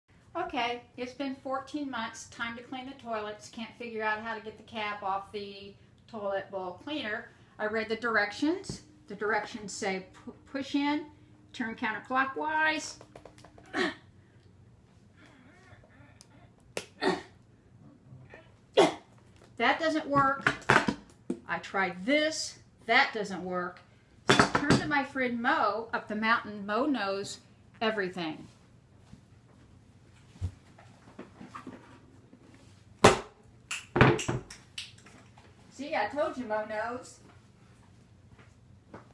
Carolbilt Video: Mo's Easy Way to Get Cap Off of Toilet Bowl Cleaner
I can't get the cap off the toilet bowl cleaner but my friend Mo can.